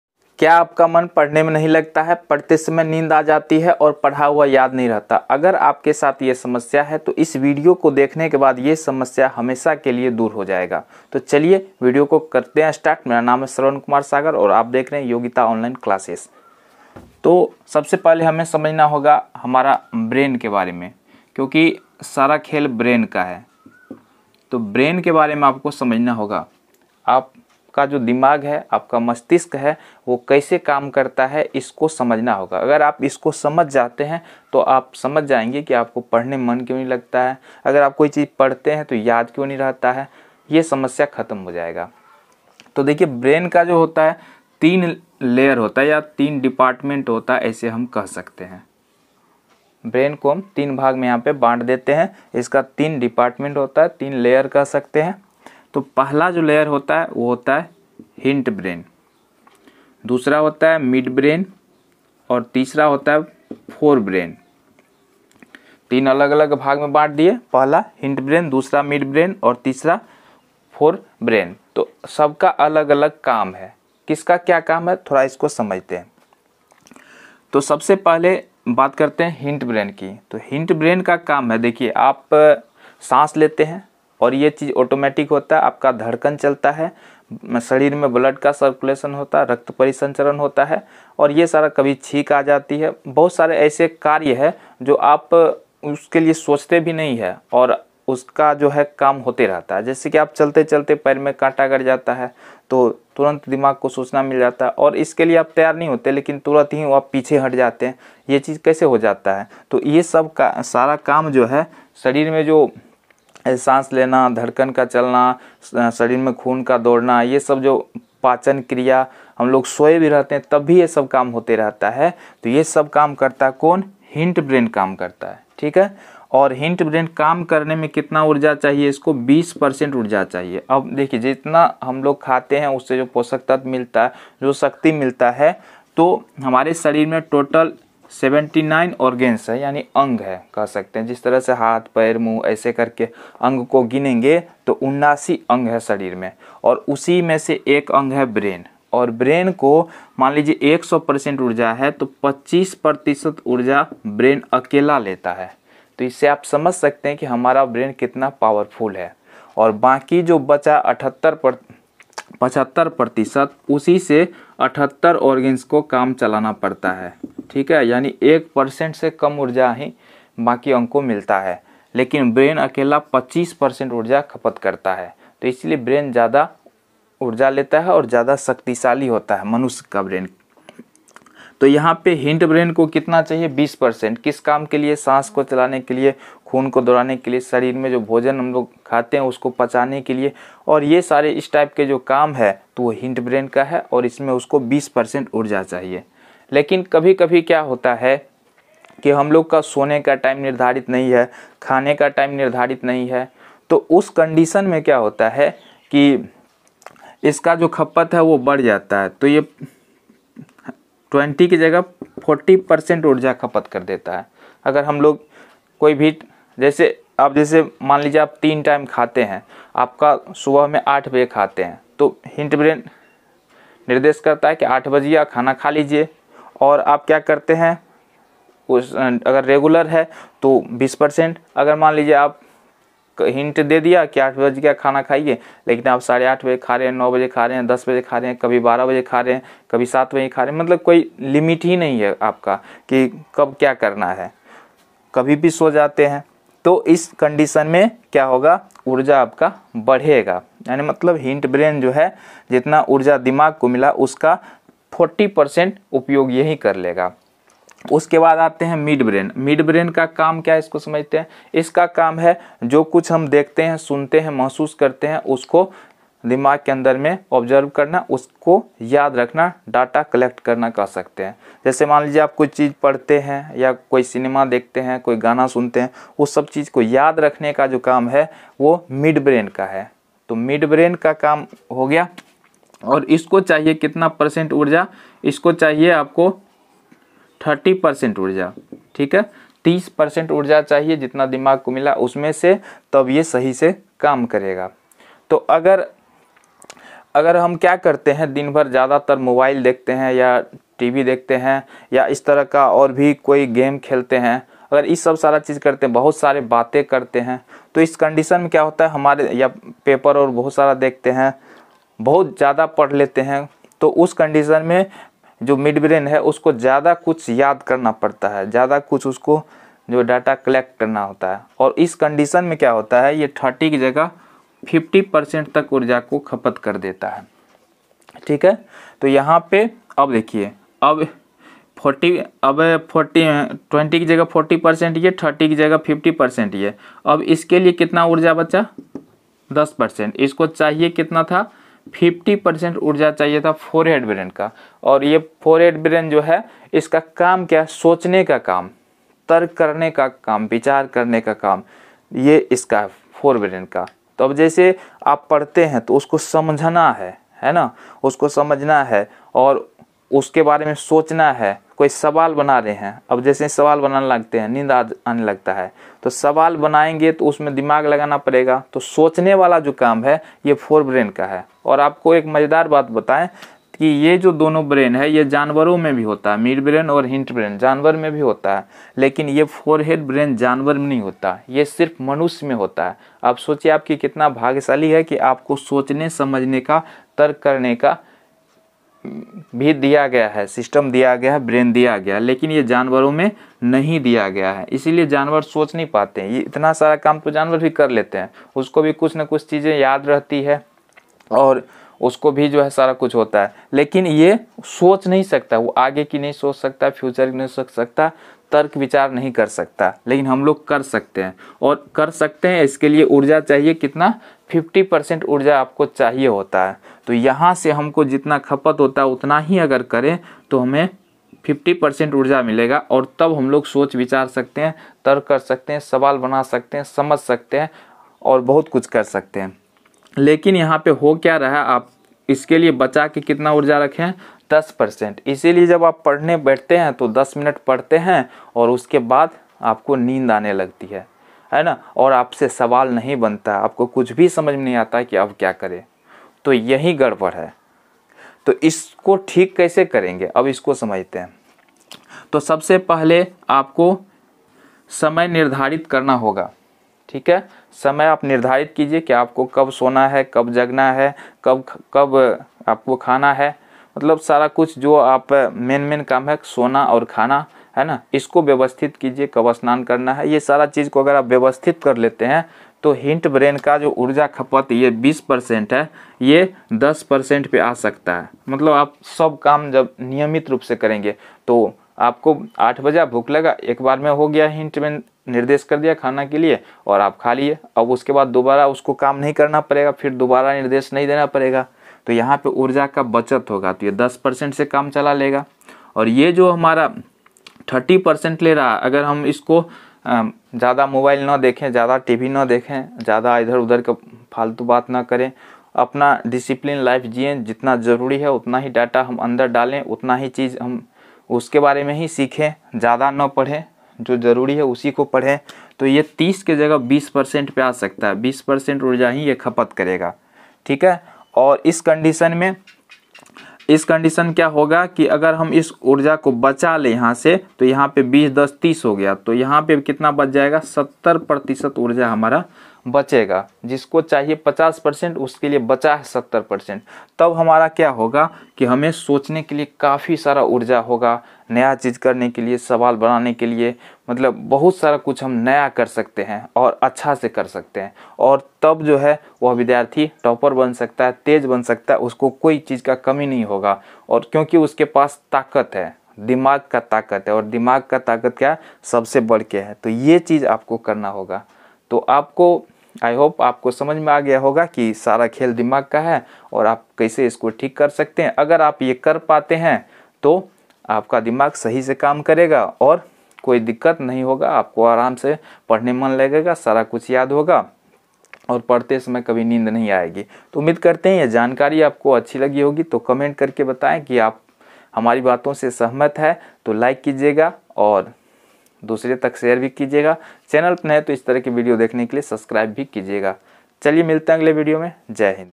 पढ़ने में मन कैसे लगाएं || Padhne Me Man Kaise Lagaye || Yogita Online Classes
Es video me maine bataya hai ki padhai me man nahi lagta hai to kya karen padhne me man kaise lagaye padhte time nind kyon aa jati hai agar aap ko video achchha lage to like kare aur channel ko subscribe karen.

pawarful motivational video
how to work our brain